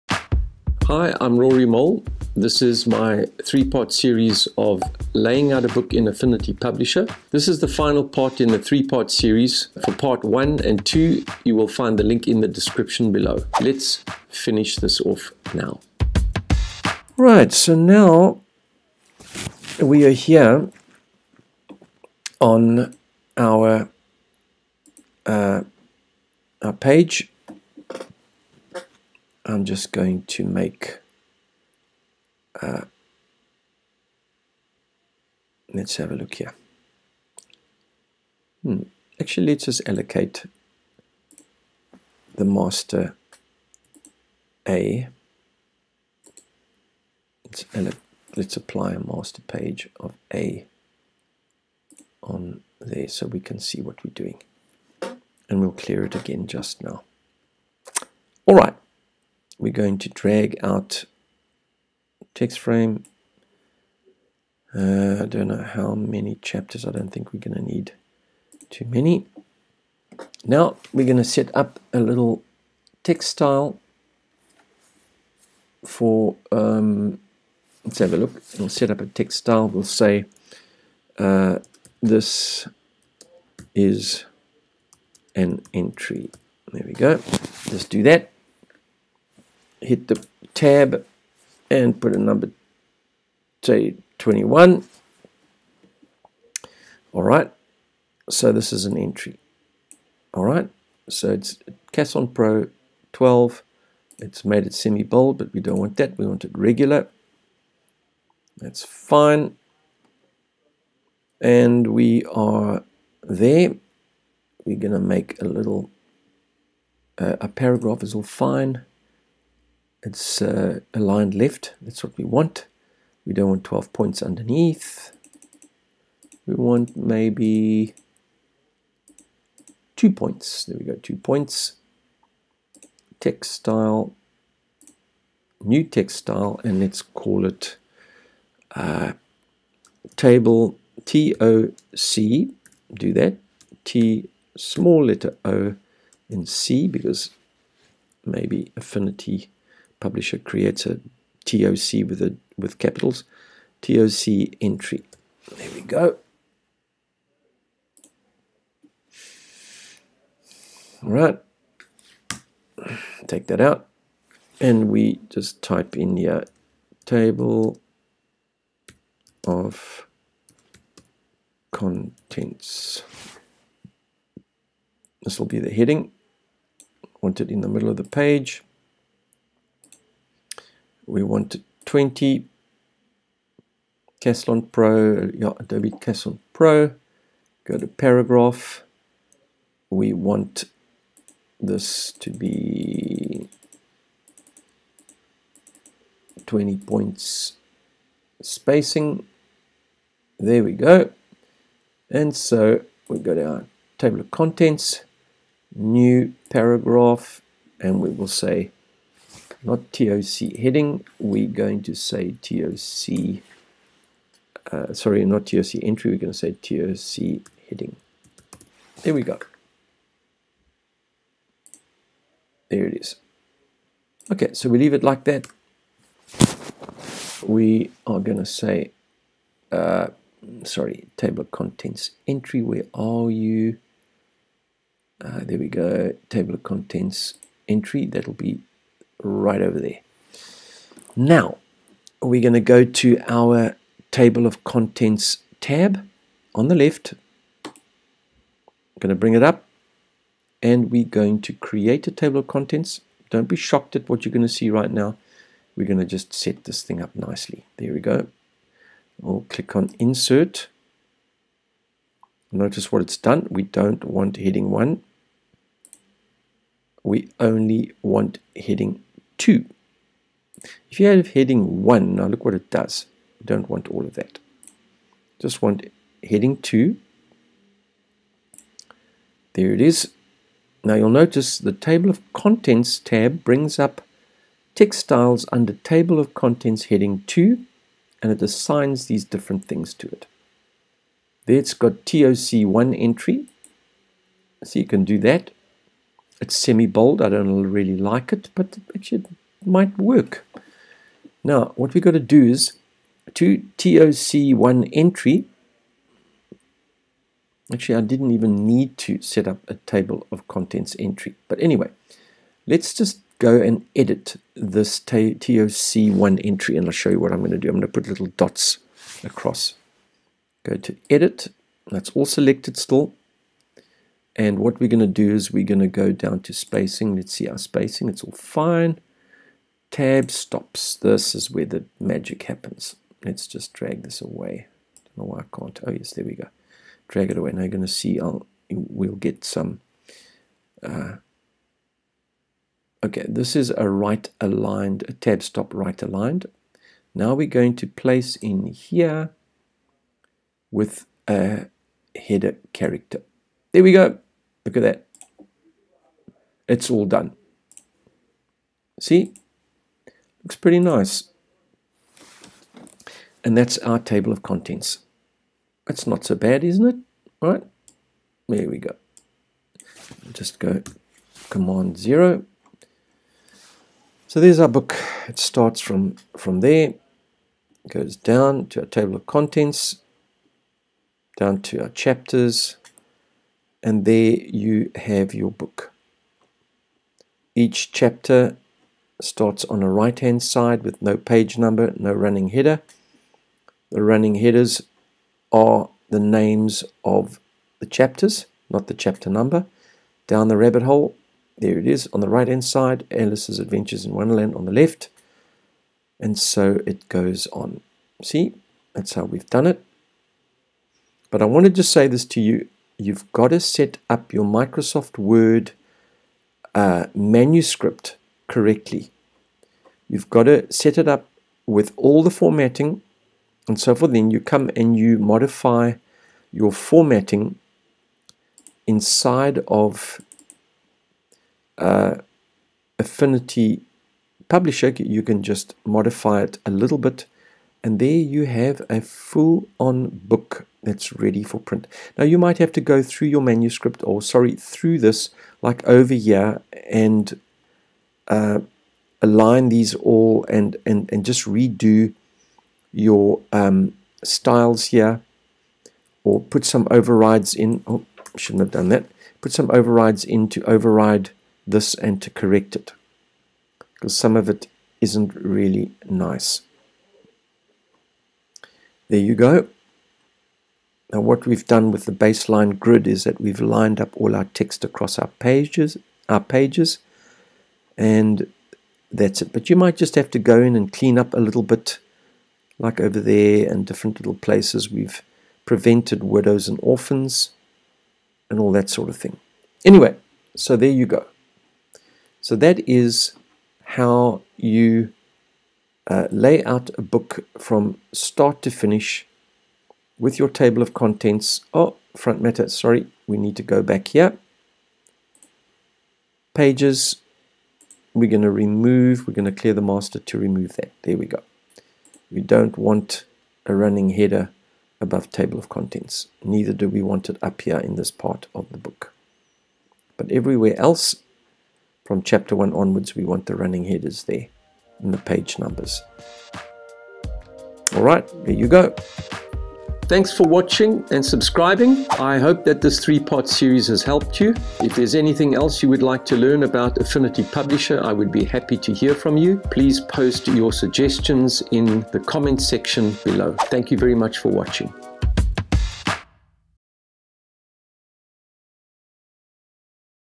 Affinity Publisher: How To Lay Out A Book (Part 3)
Part 3 of laying out a complete book in Affinity Publisher, from start to finish.